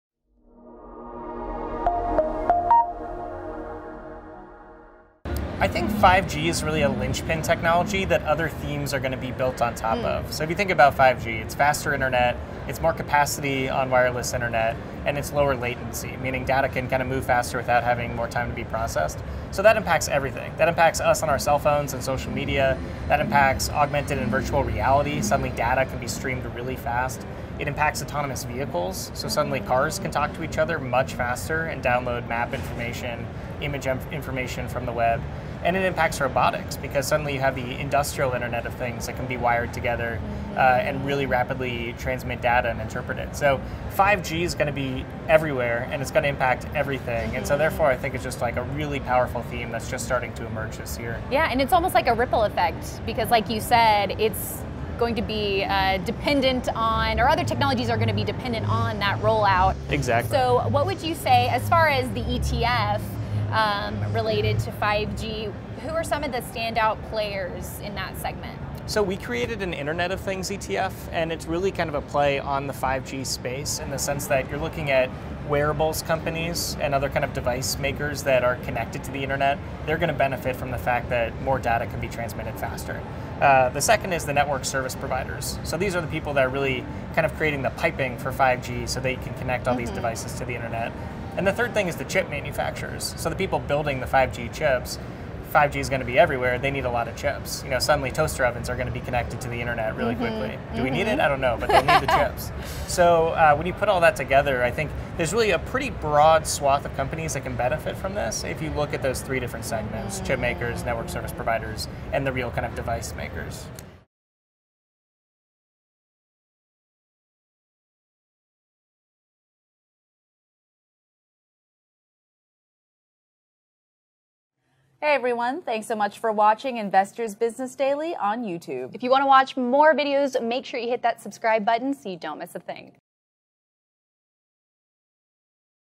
This Is Why 5G Is A ‘Linchpin Technology’
According to Jay Jacobs, head of research at Global X, the 5G wireless network is set to have a notable impact on chipmakers, network service providers and device makers.